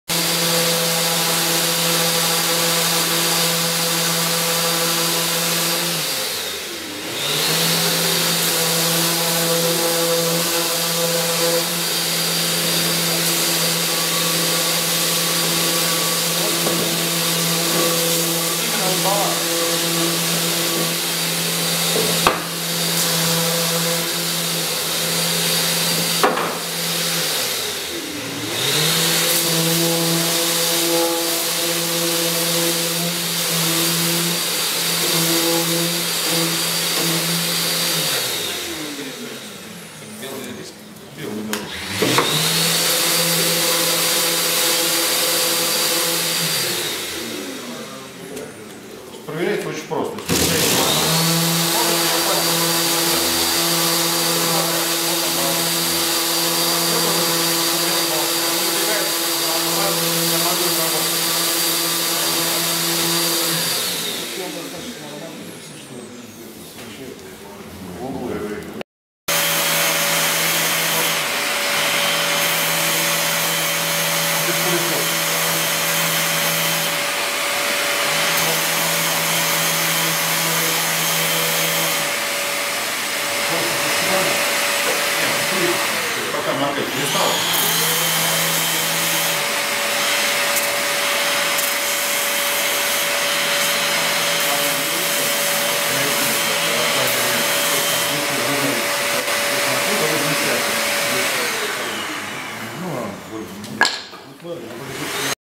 Шлифовальные машинки MIRKA
Демонстрация инструмента MIRKA в компании "Арсенал Мастера" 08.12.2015. Работа шлифмашинками MIRKA